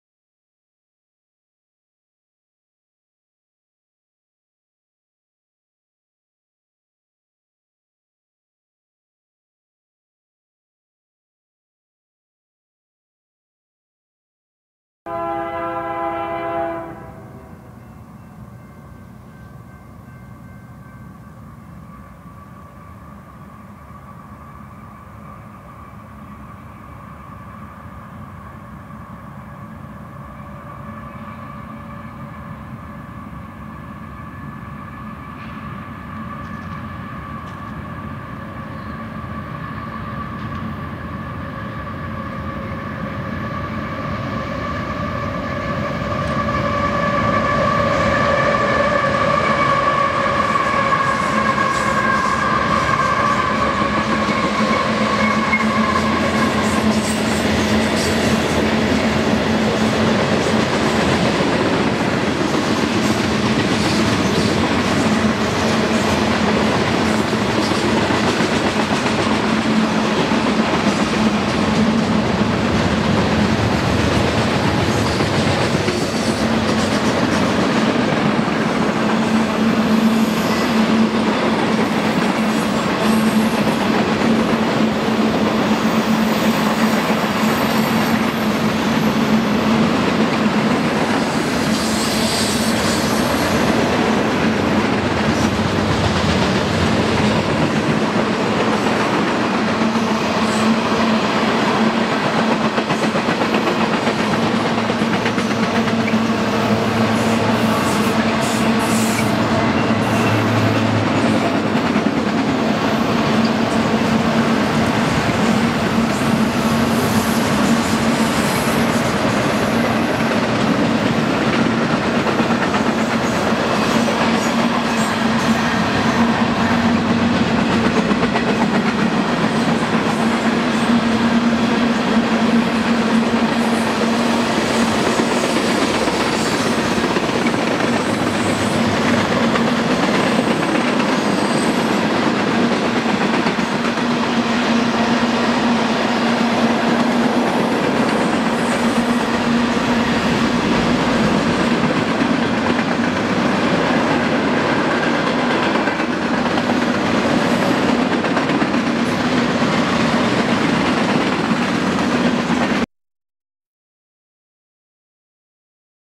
UP 6863 C44AC GE Manifest Train BNSFRon D
BNSF Railfanning:   UP 6863 C44AC GE  Manifest Freight Train at MP15 heading East Bound thru Barstow, Ca. on to Yermo, Ca. and then to Las Vegas, Nev.